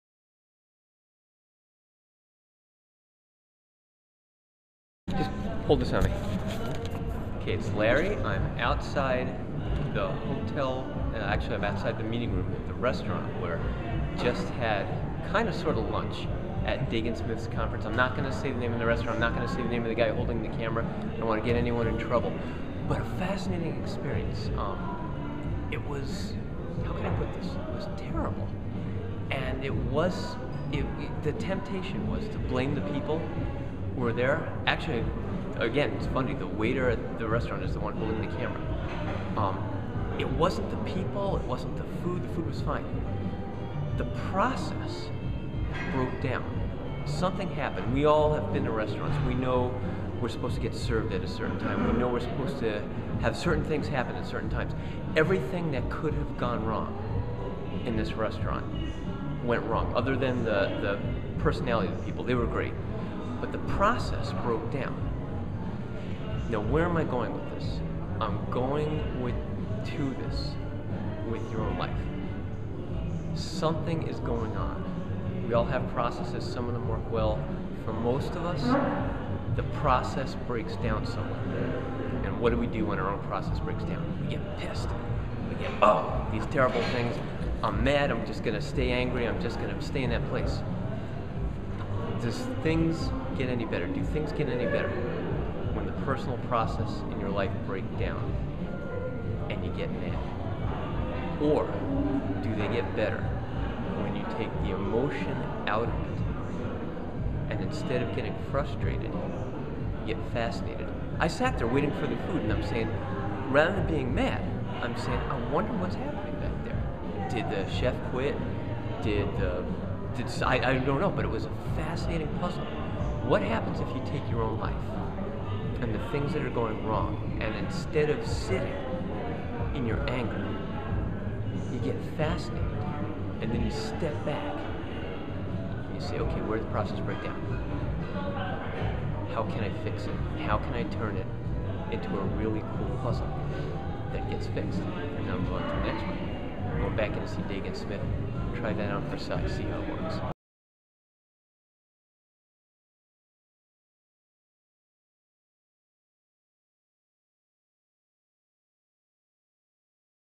How To Attract: Don't Get Mad, Get Fascinated!
Sorry for the darkness, but this is one of those "heat of the moment" videos.

We had just gotten out of lunch break at Deagan Smith's Maximum Leverage seminar in Baltimore. The restaurant was absolutely horrible.  Service was terrible, food was so-so.  No one seemed to know what to do, and didn't know enough to fix it.

Instead of getting angry, I got fascinated about how things went so wrong. My emotions shifted to trying to solve a difficult puzzle rather than being mad because my oyster po-boy still hadn't come out after an hour.

What happens if you take your own anger, frustration and disappointment and shift it to fixing the puzzle of your own mess?

You have a really good chance of fixing it.

And you get very attractive to people who like your energy.

Give it a shot, Dude!